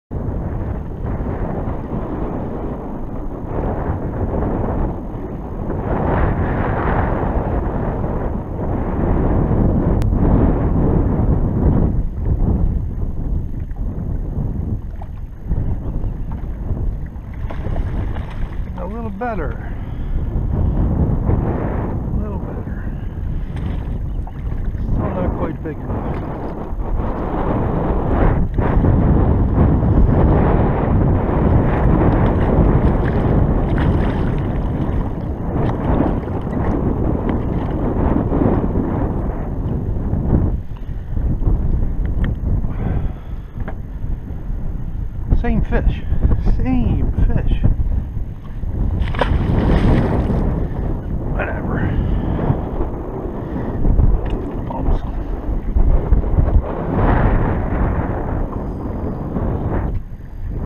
Striped Bass For One Minute
Short Striped Bass everywhere and not a fish to eat